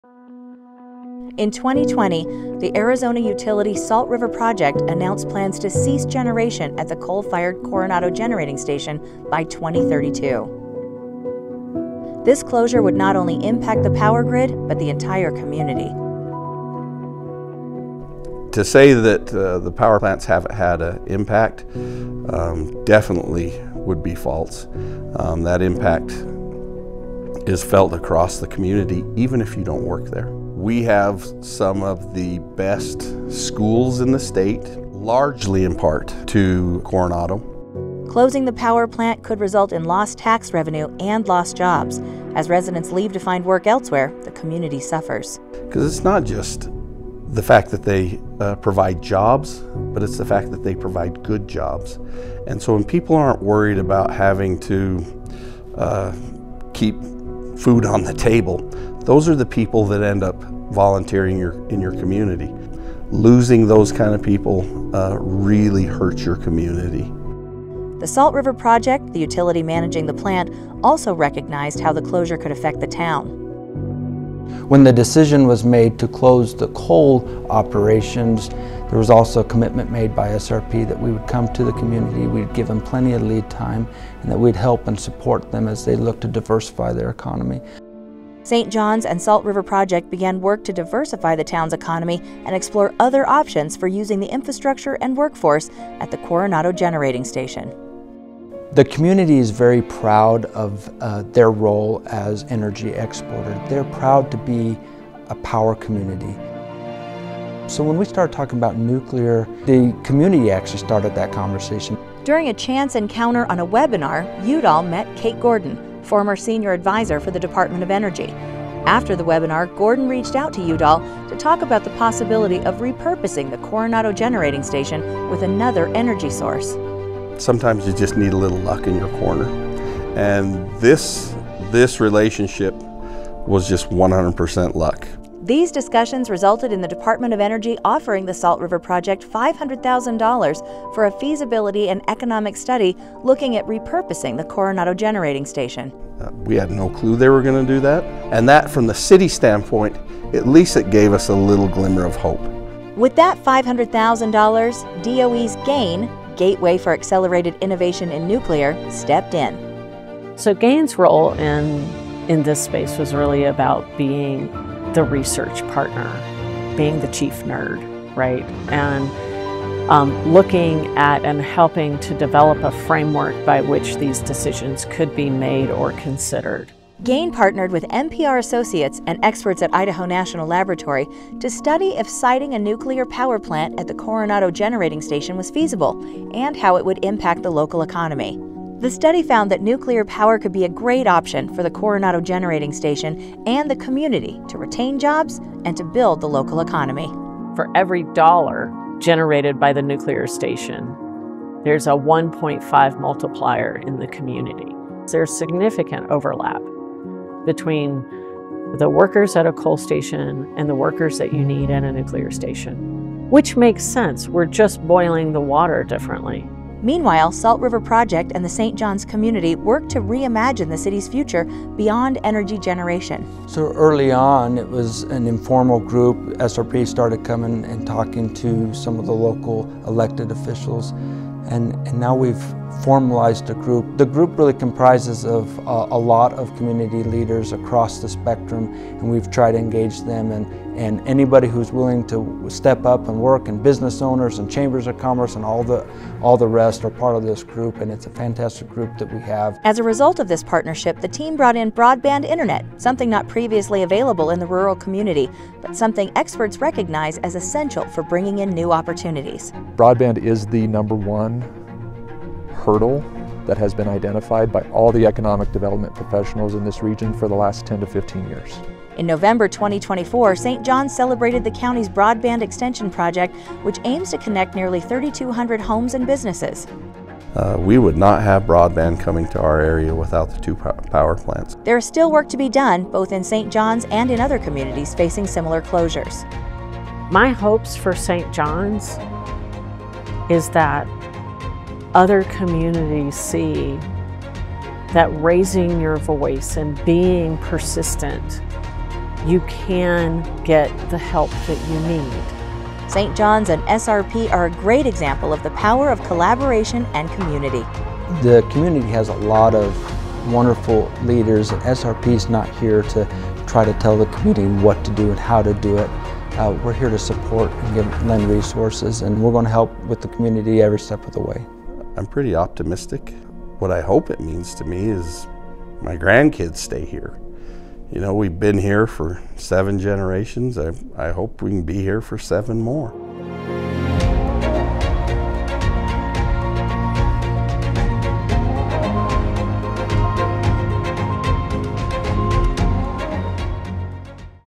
St. Johns, AZ: A Town’s Journey Through Change | GAIN Community Support
What does a town do when faced with the proposed closure of its main employer?

This was the dilemma facing St. Johns, Arizona, when Salt River Project announced plans to cease operations at the coal-fired Coronado Generating Station by 2032.

In response, the town came together to build a stronger future. Efforts included extending the region’s broadband internet and collaborating with the U.S. Department of Energy’s Gateway for Accelerated Innovation in Nuclear (GAIN) to assess the feasibility of nuclear power for the plant and the community. The DOE-funded study concluded that nuclear power could be a viable option for the Coronado Generating Station, providing job retention and economic growth for the local area.